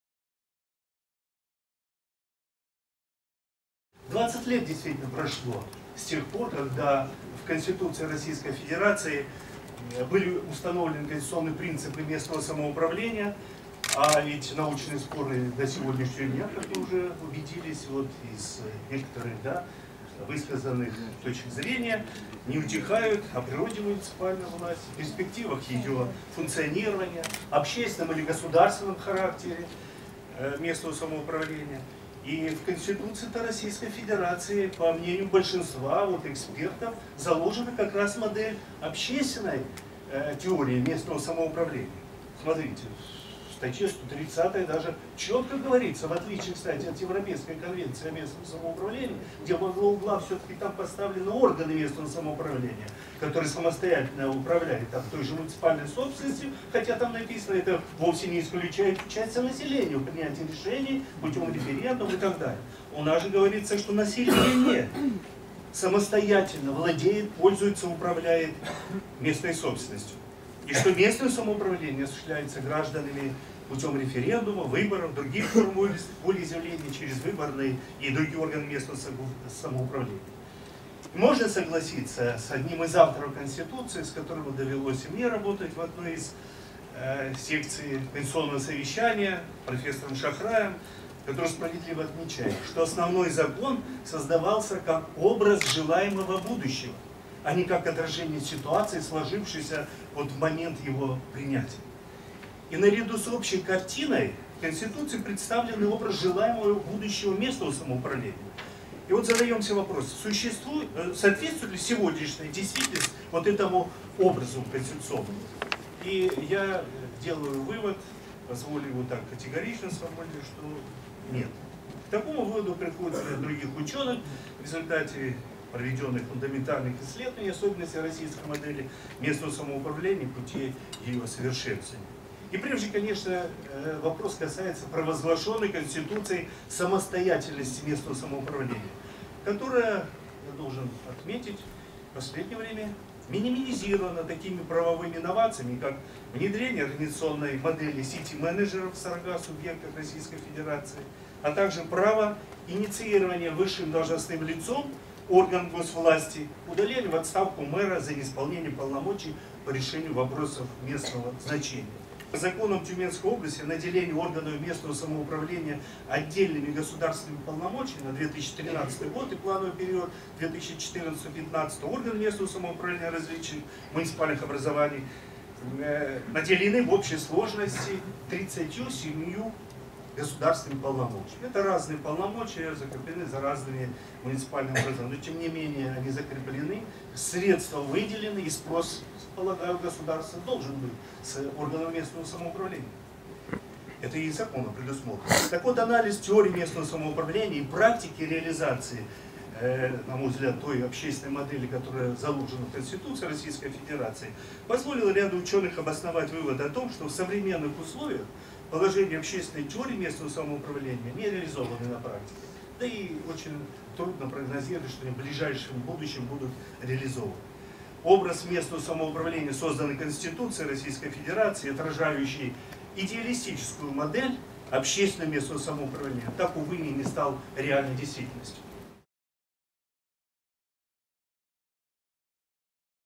Геннадий Чеботарев
Геннадий Чеботарев, президент Тюменского государственного университета, заслуженный юрист России, доктор юридических наук